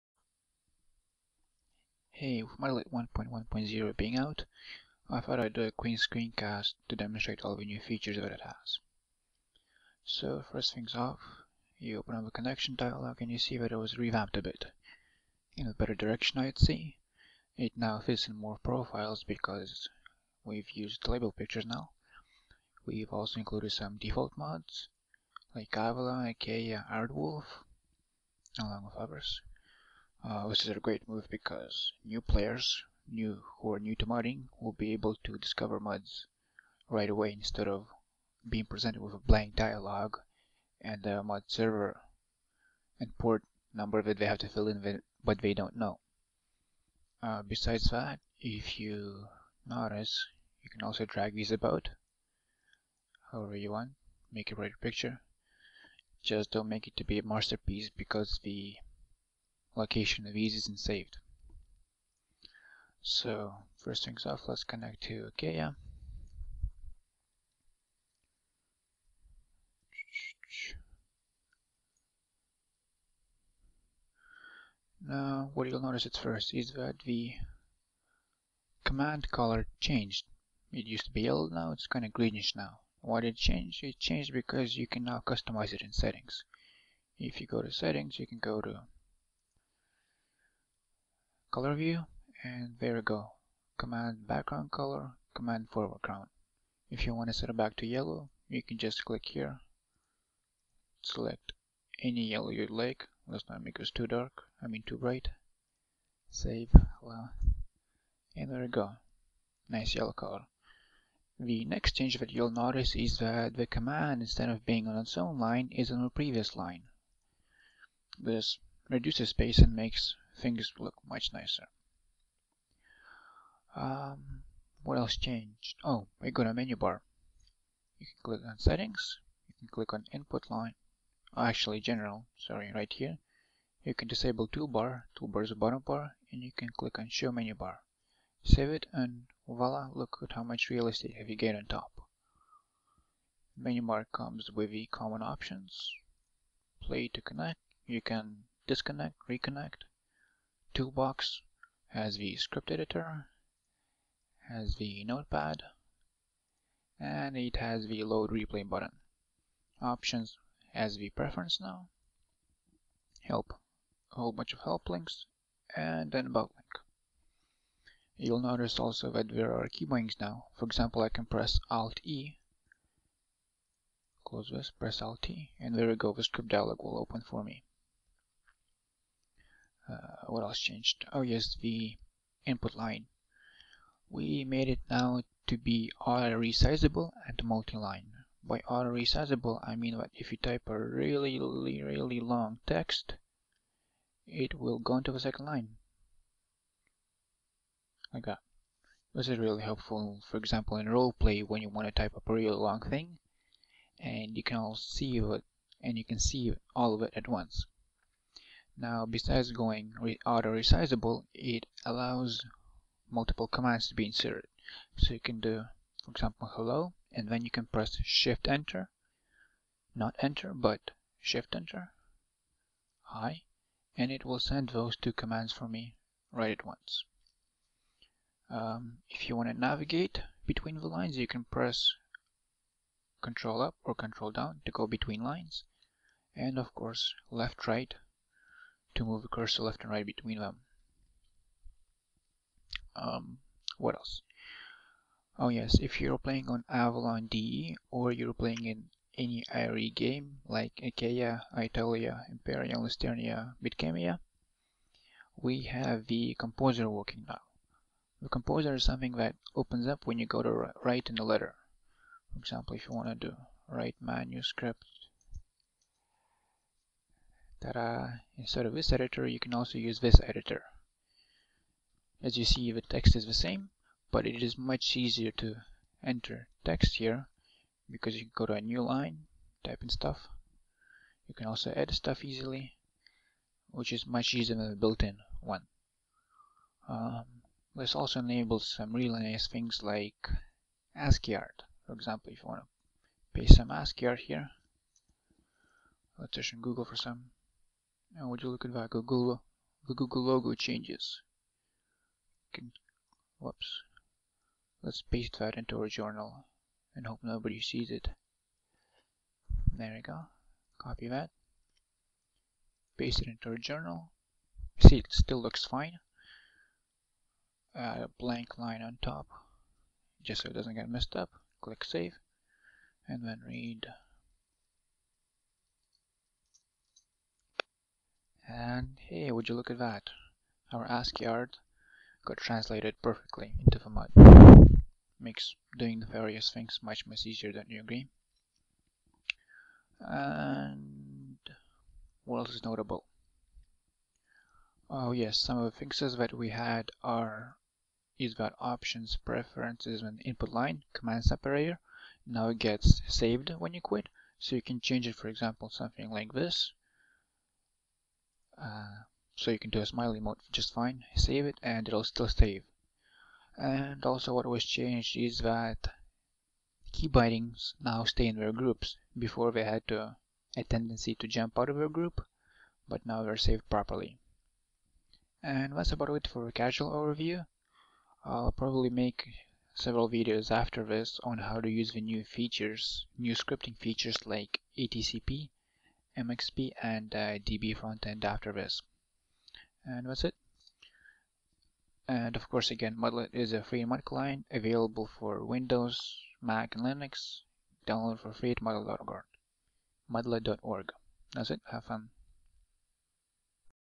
Casual overview of Mudlet 1.1.0
New features available in Mudlet 1.1.0, overview without scripting.